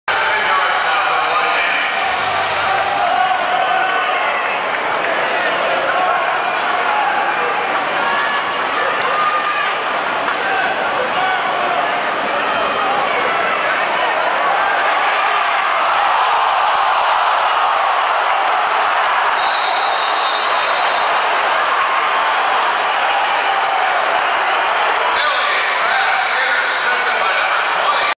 Stockton Lightning Football 2008
Our team got the ball intercepted, but the dude fell out of bounds trying to do it. Nice